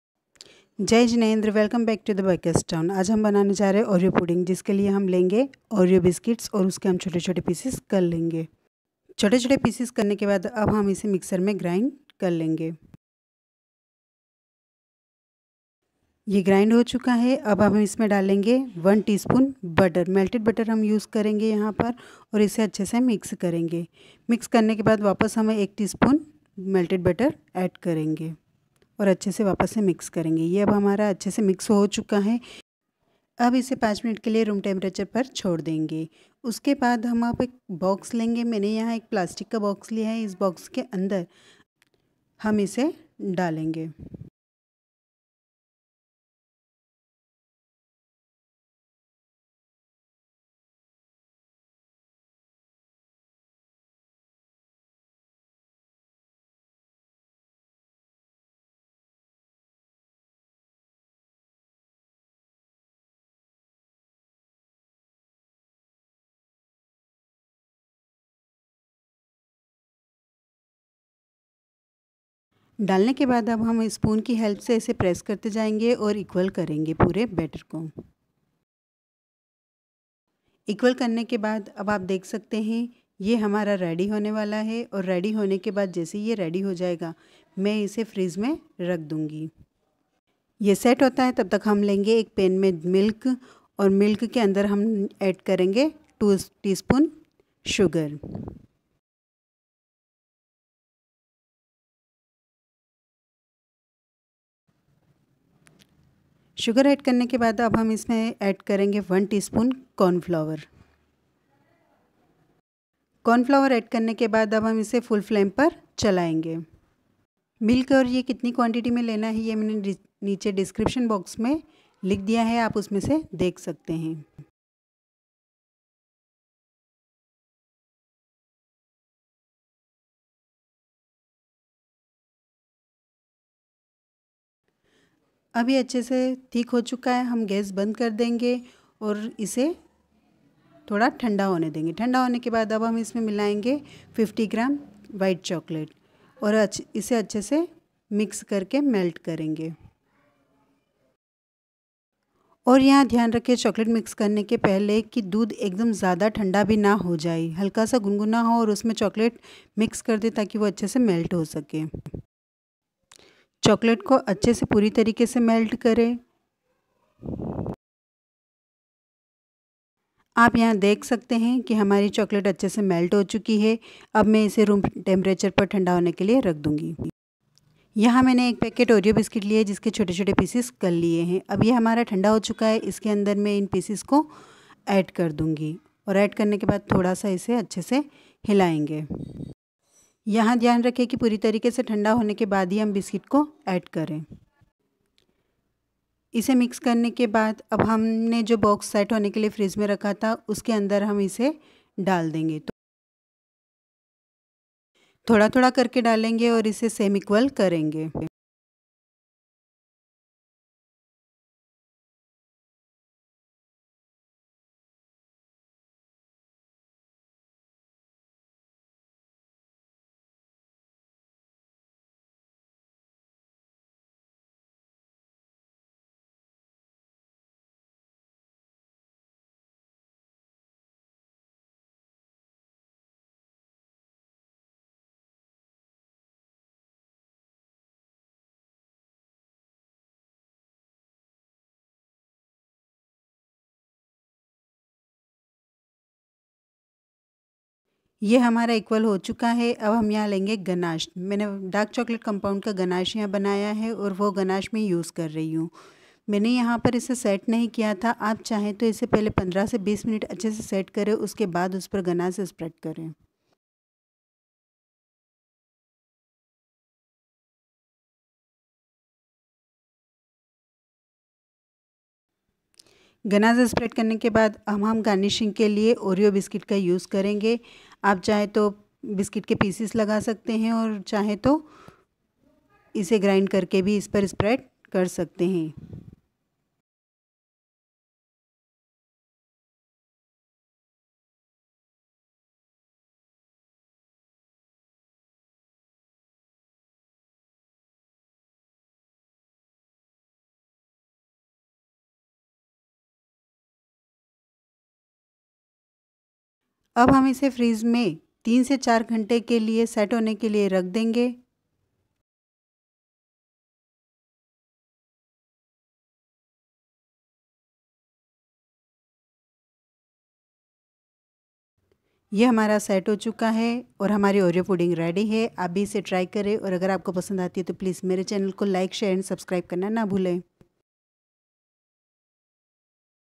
oreo pudding
ingredients
oreo -5pkt
butter-2tsp
milk-250ml
sugar-2tsp
corn flour-1tsp
white chocolate-50gm
ganashe-75gm

for ganashe - dark chocolate- 50 gm
                        fresh cream-25gm